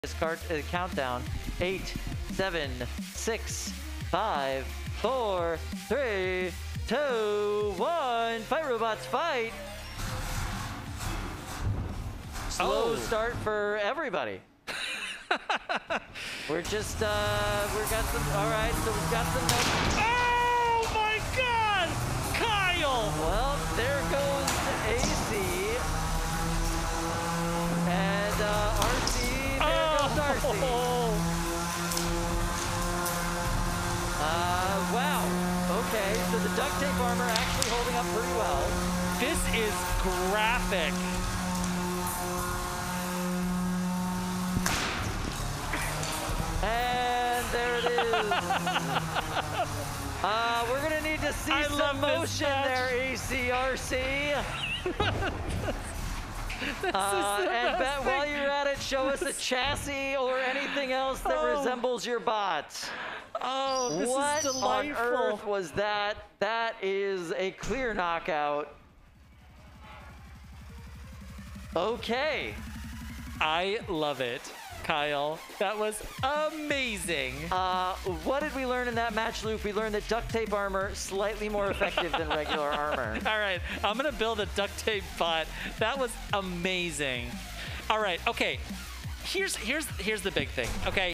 NHRL Mar 2021 - AC/RC vs Pramheda - 12lb
Our most destructive fight of the day.